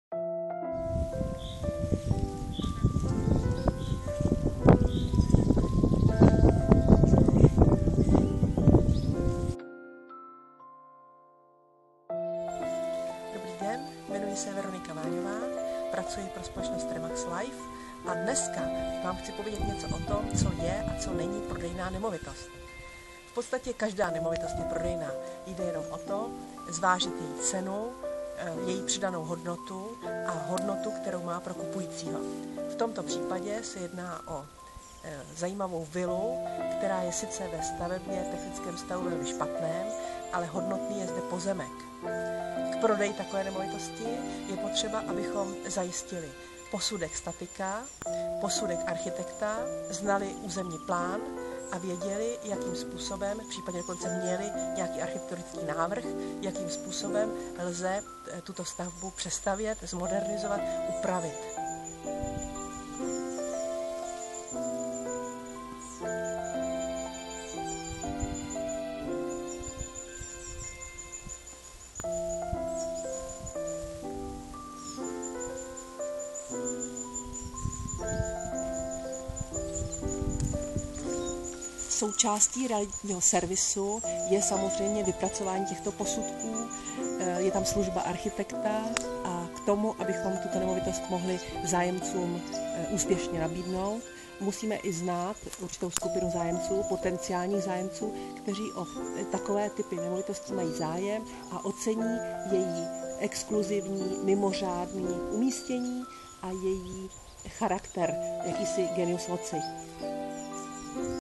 O prodeji starého domu.mp4
Víte, jak lze prodat i starý a poškozený dům ? Přijďte se poradit s Veronikou.